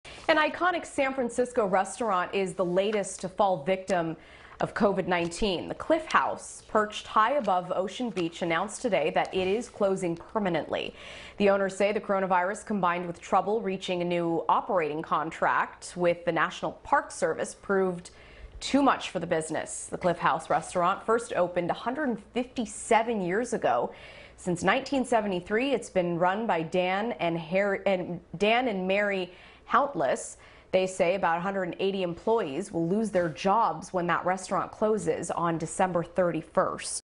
Cliff House Restaurant in San Francisco to Close Permanently
The Cliff House restaurant, which first opened 157 years ago, will close permanently on Dec. 31.